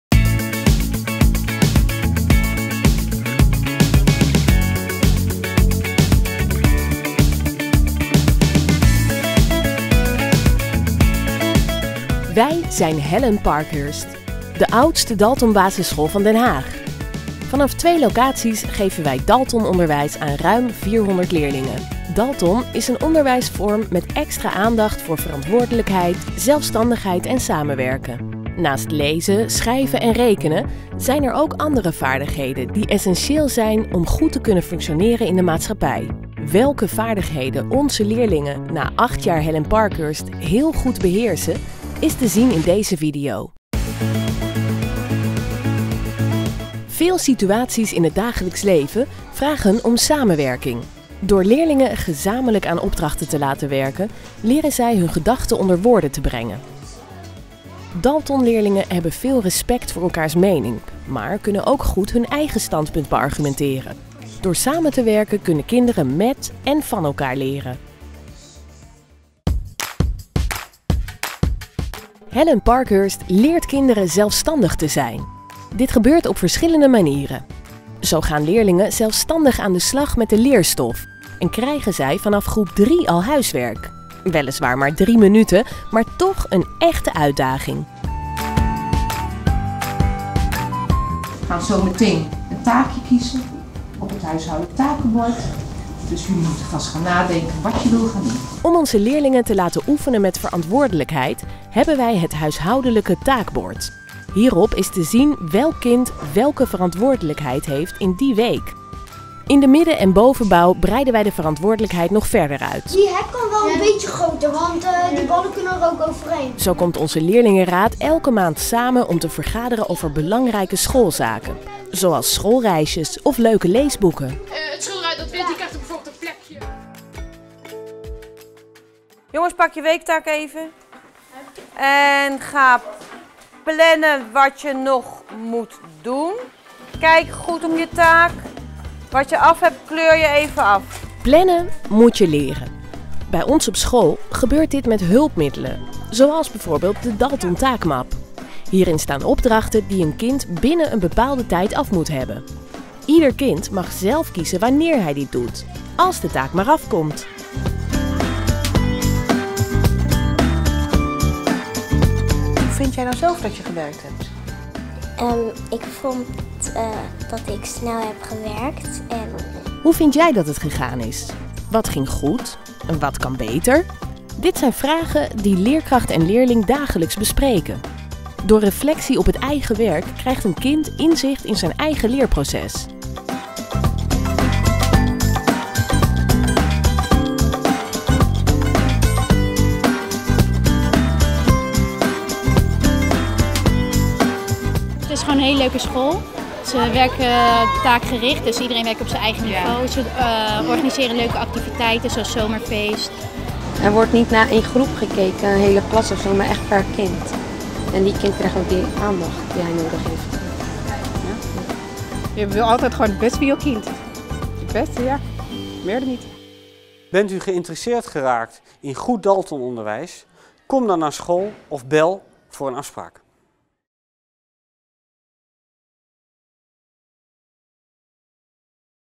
Daltonschool Helen Parkhurst Den Haag
Dit is het promotiefilmpje van Daltonbasisschool Helen Parkhurst in Den Haag. Wilt u meer weten?